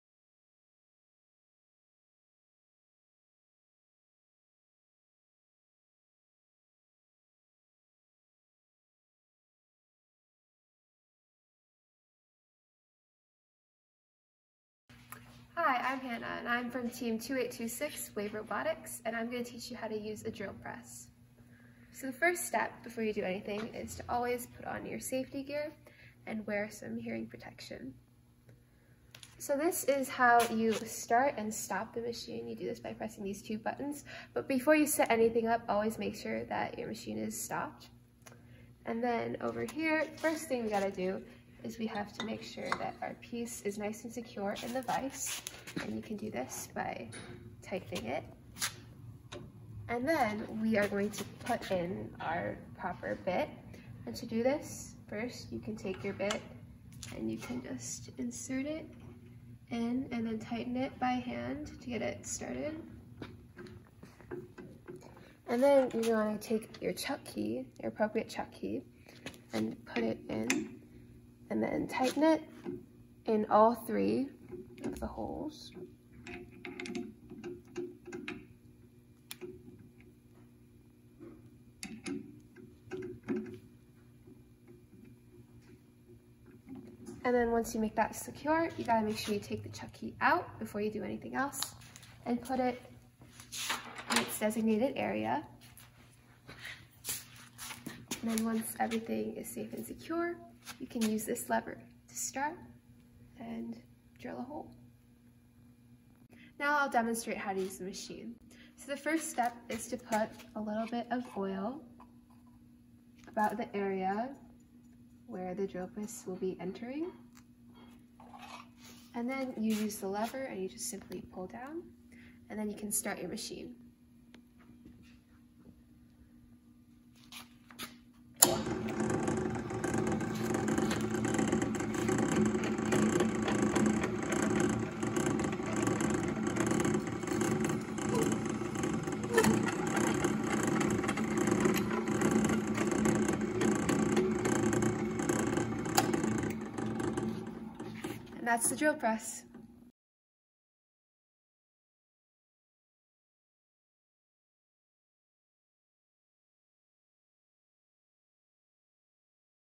Drill Press
FRC team 2826 Wave Robotics Safety Training Videos: How to operate the drill press.